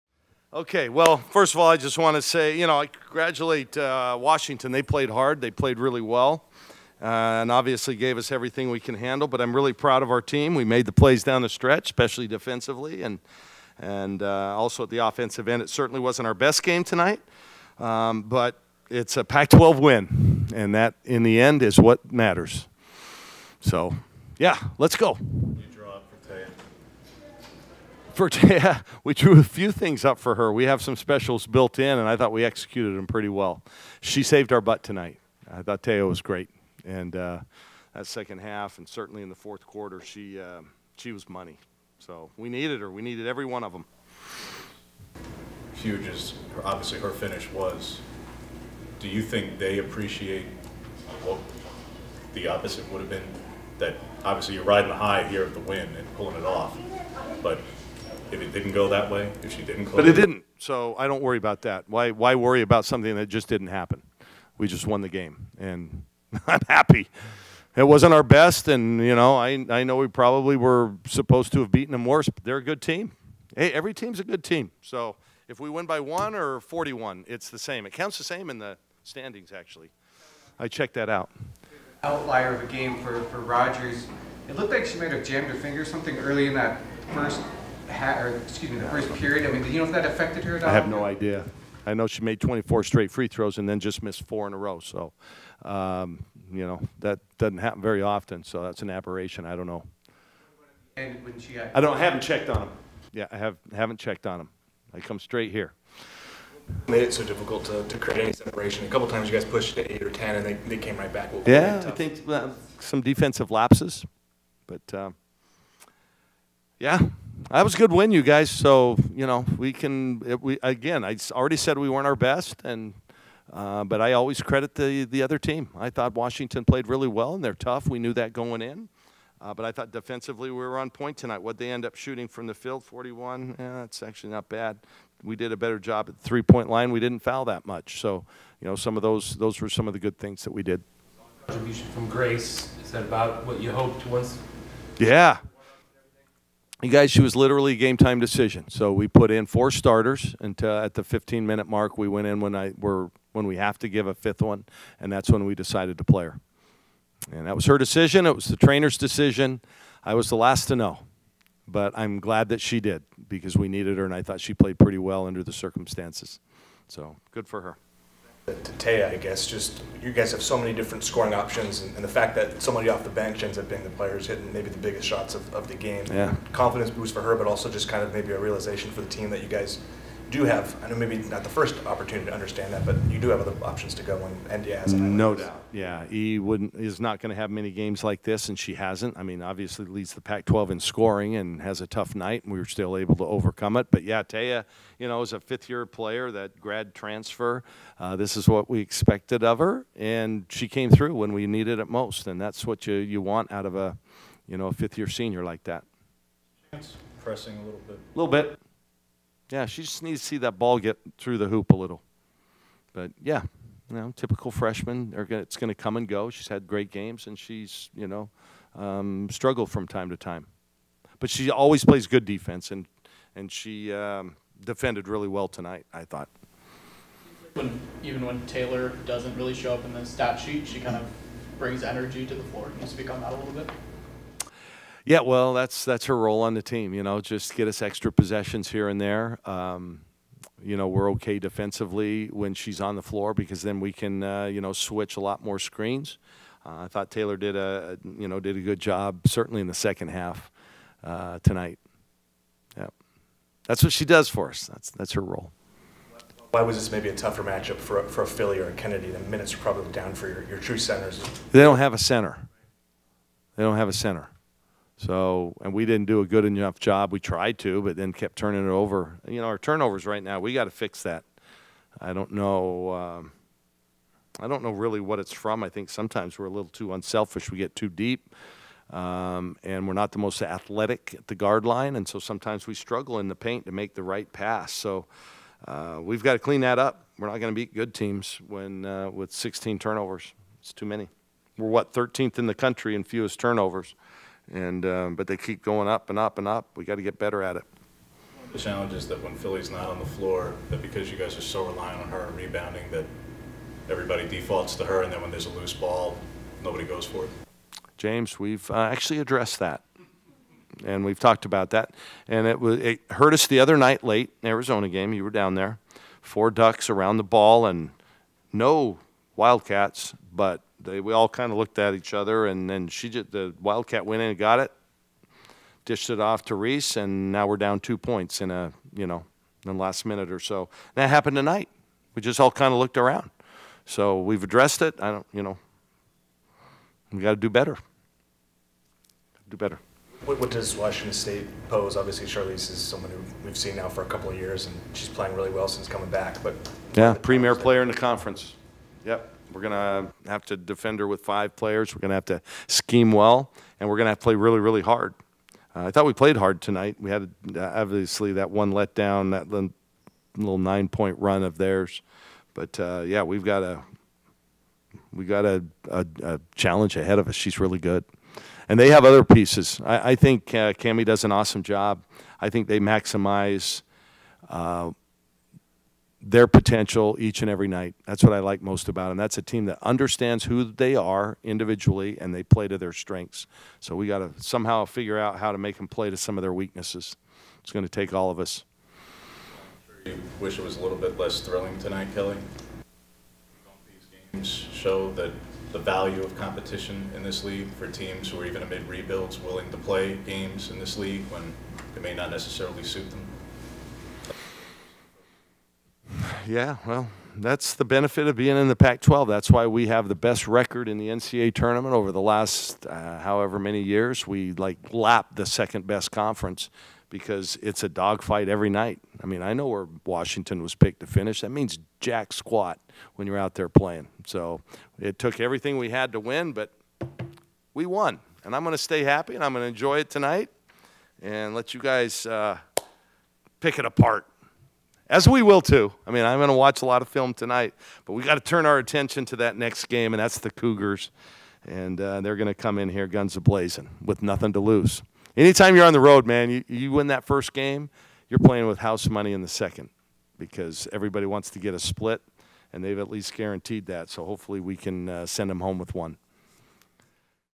Kelly Graves | Postgame vs. Washington
Head Coach Kelly Graves meets with the media following Oregon's tough 65-58 win over Washington.